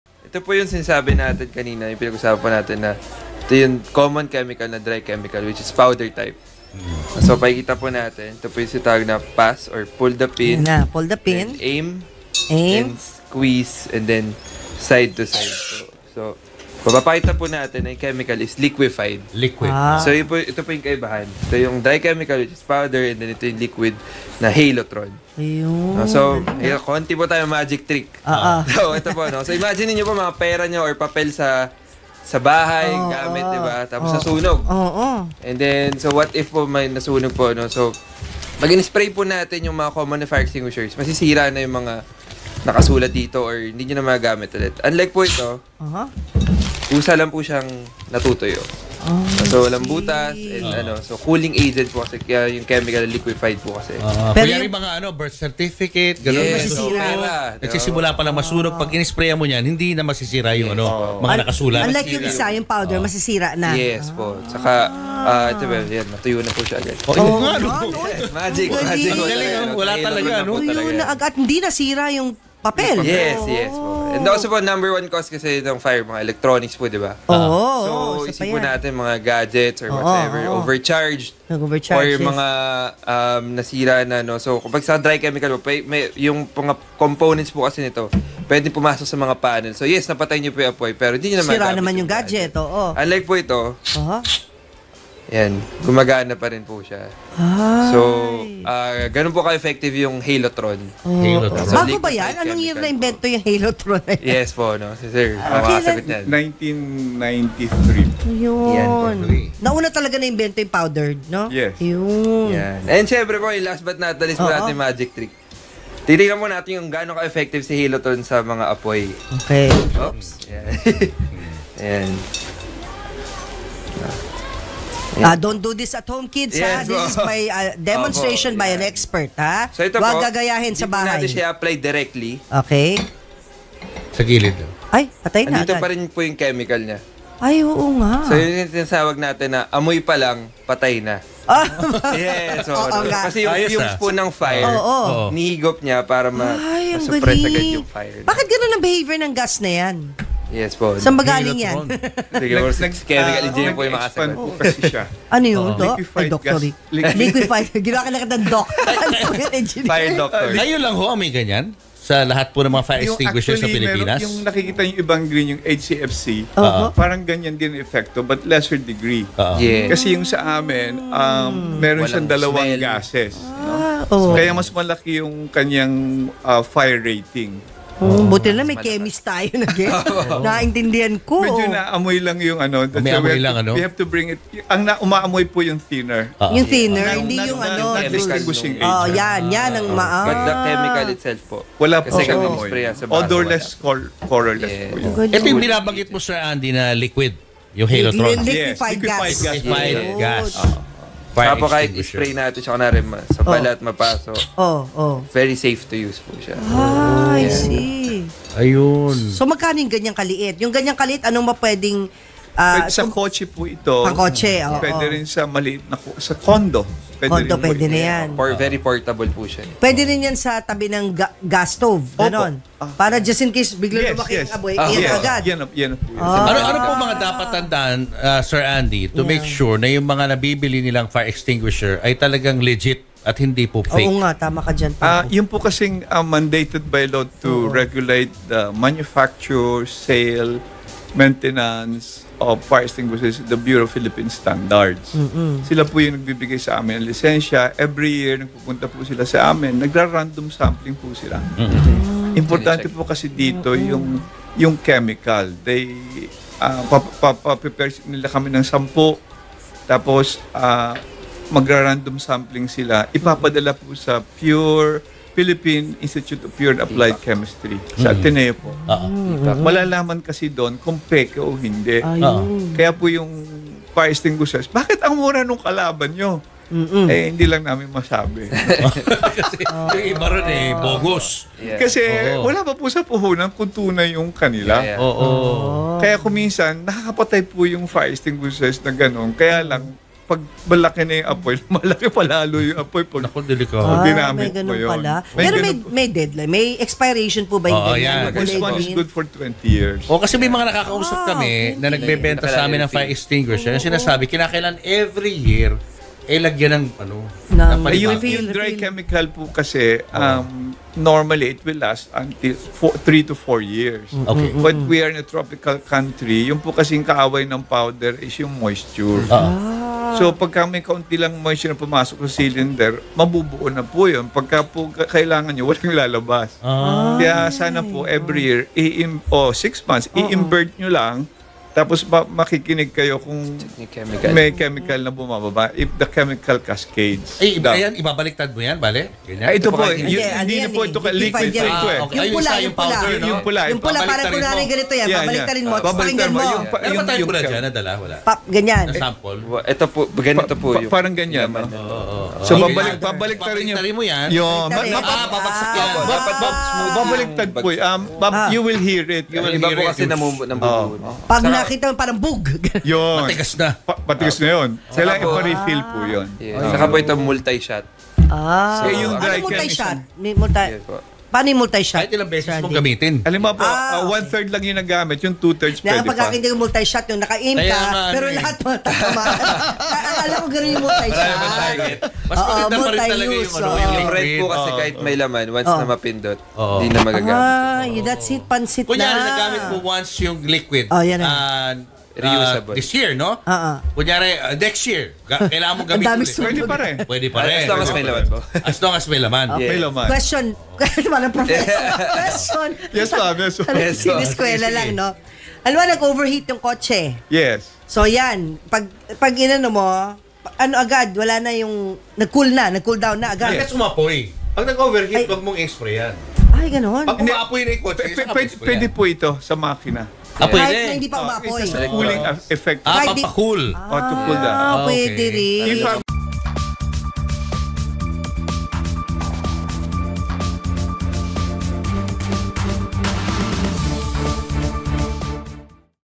JNT | Fire extinguisher na hindi nakasisira ng gamit
KAKAIBANG FIRE EXTINGUISHER

Sample demo ng fire extinguisher na hindi nakasisira ng gamit. May chemical din ito na mabilis matuyo.